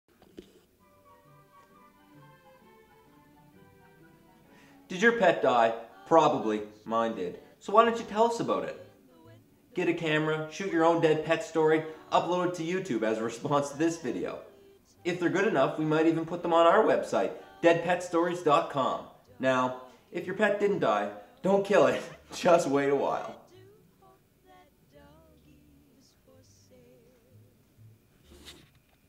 Dead Pet Stories: make your own
Zac Barkhouse shows the folks at home how to make there own "Dead Pet Story". Give it a try.